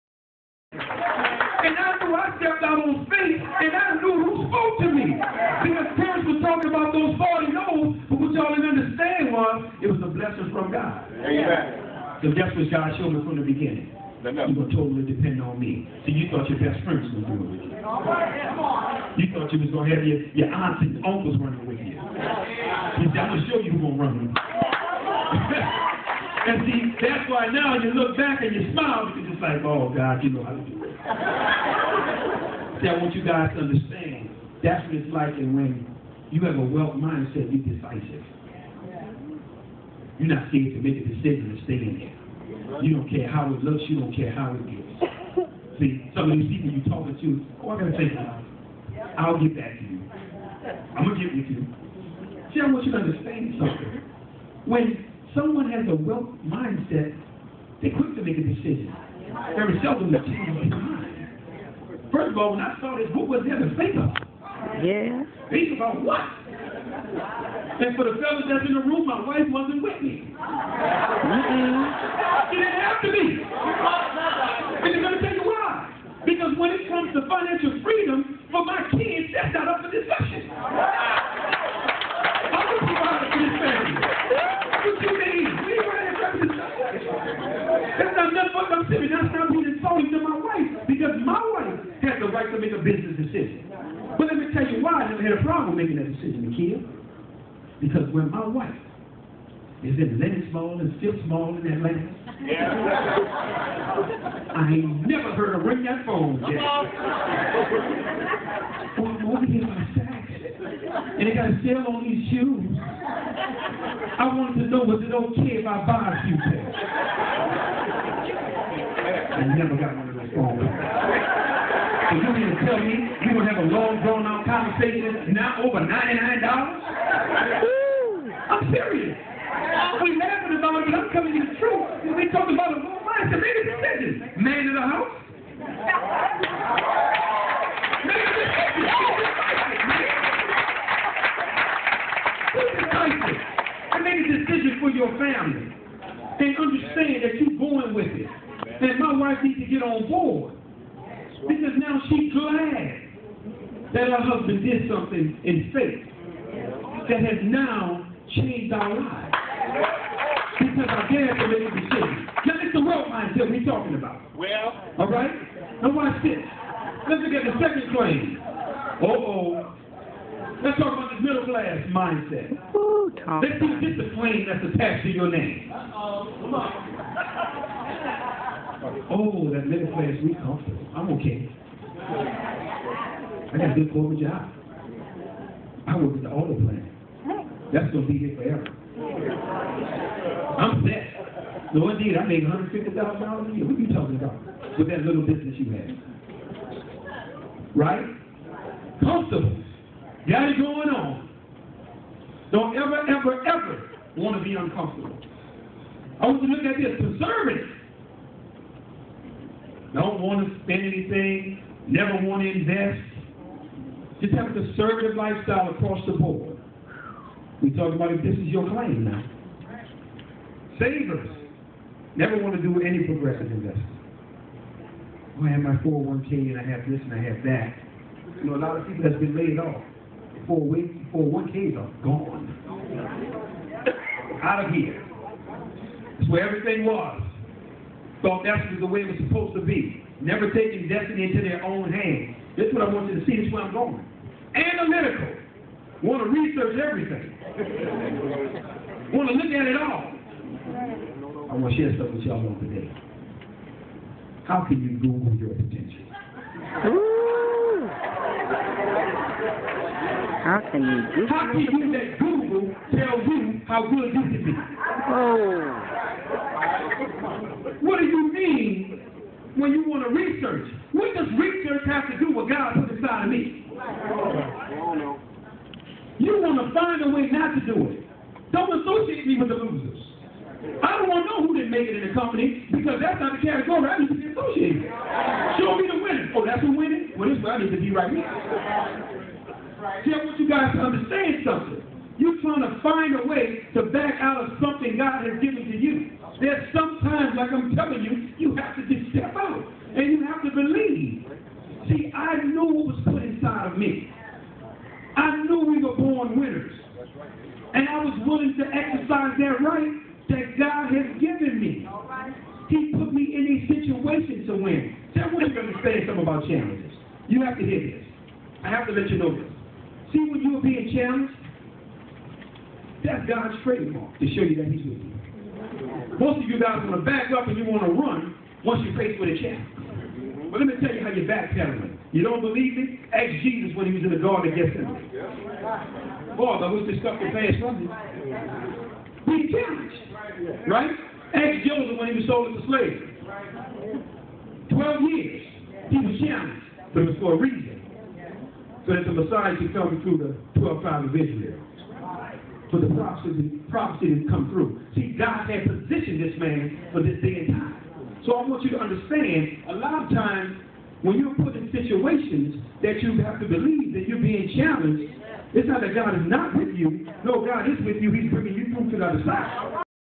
Demond Crump, Hurricane Katrina Moved Him to ZAMZUU!
From the ZAMZUU NATIONAL HEROES TOUR KICKOFF IN DEARBORN MICHIGAN, ON MAKING DECISIONS. The first 40 people he shared Zamzuu with said "NO" but now he is one of the top earners in Team Vision among $100,000 in 12 month period ring wearers. .